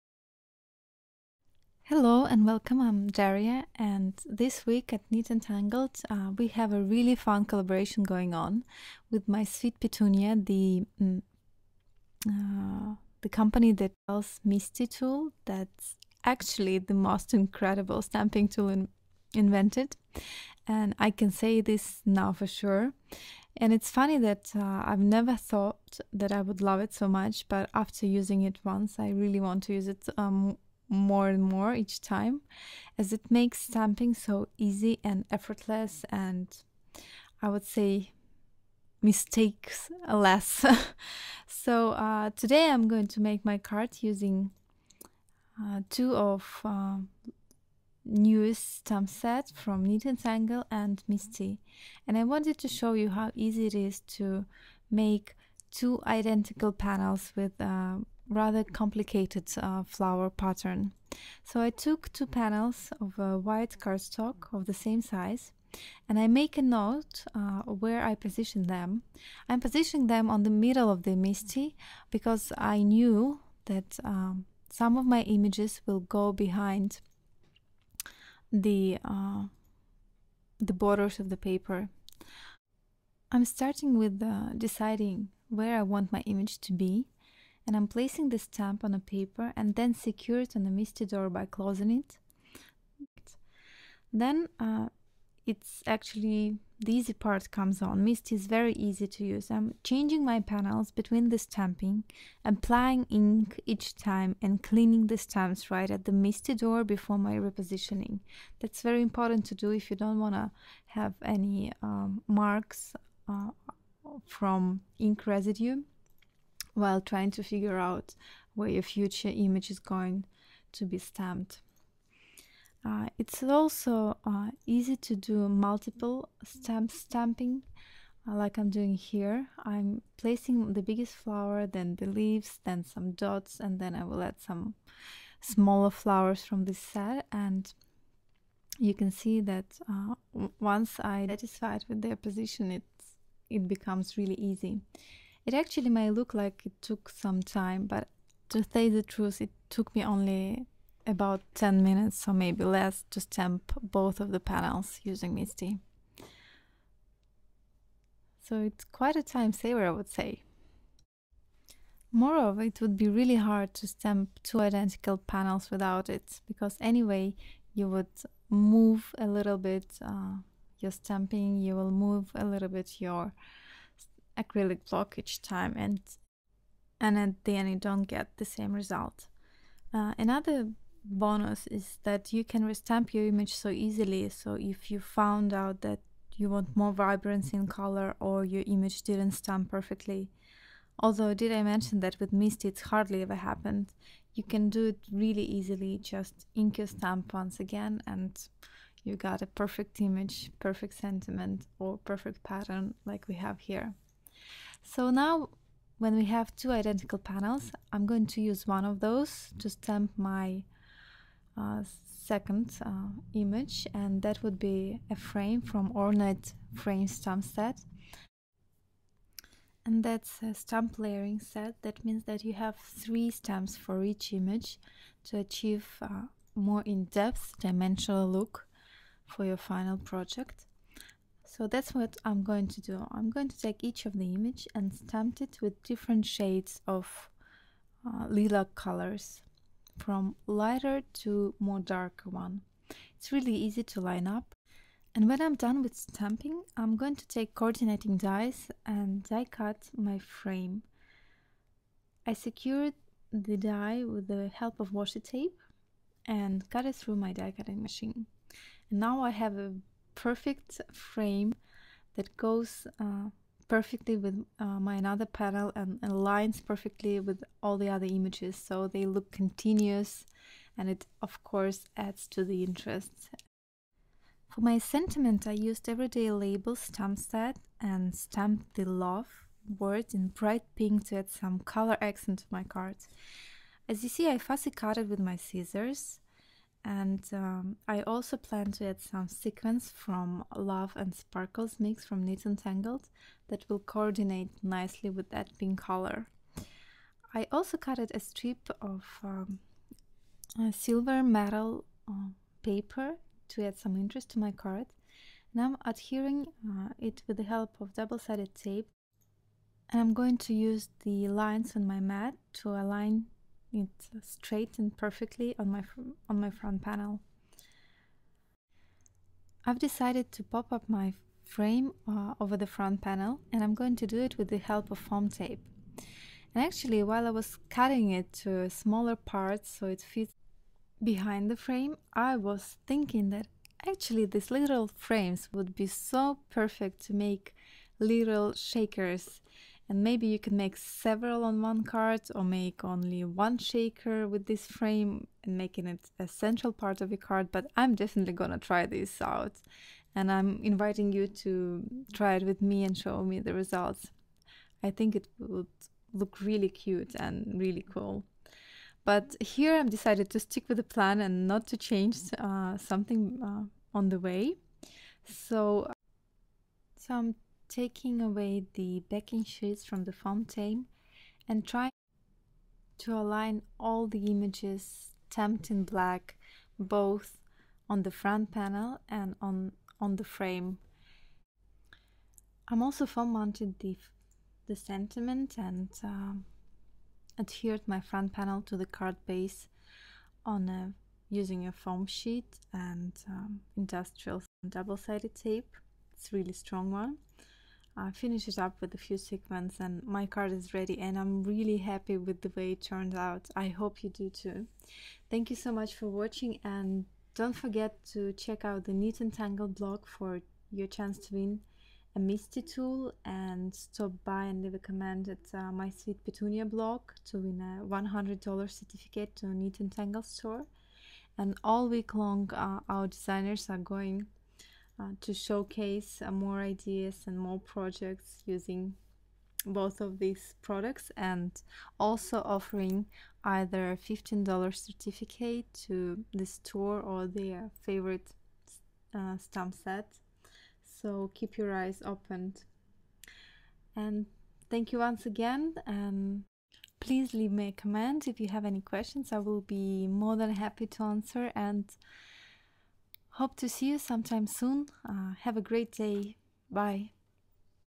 (ENG) Neat&Tangled: Love in Frame Card for collab week with MISTI
Featuring MISTI tool from My Sweet Petunia and two stamp sets from Neat&Tangled: So Good to See You and Ornate Frames.